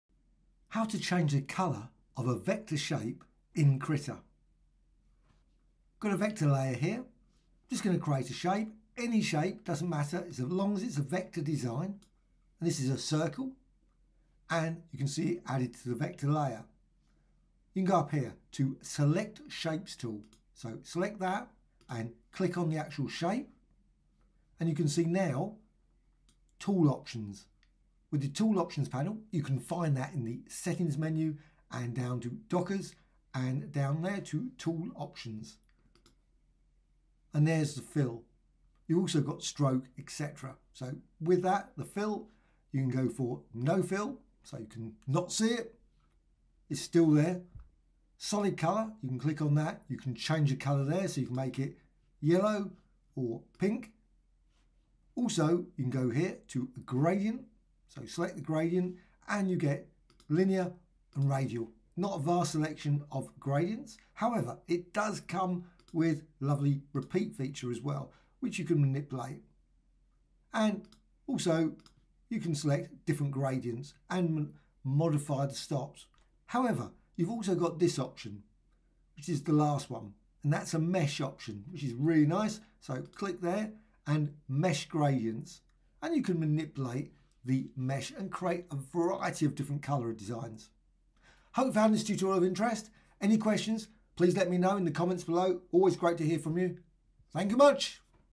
Krita Tutorial : How To Change Fill Color Of Vector Shapes (4)
Krita : How to change / modify the fill color of a vector shape (circle, polygon etc) in Krita